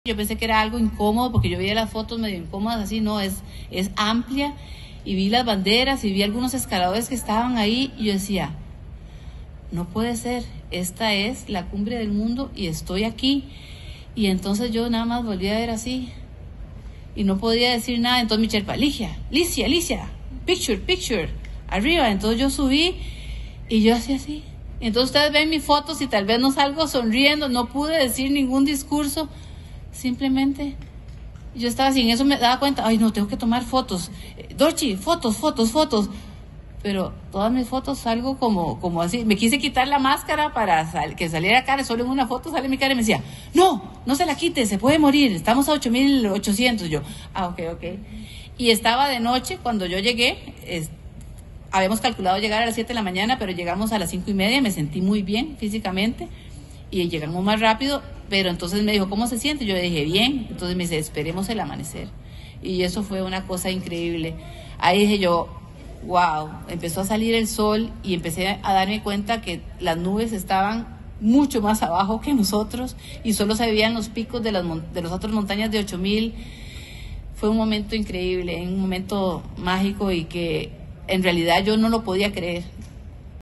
Ligia Madrigal relata cómo es la cima del Everest
Ligia Madrigal relata cómo es la cima del Everest. Ella llegó a la 8.848 metros sobre el nivel del mar el 22 de mayo y se convirtió en la primera mujer costarricense en lograrlo.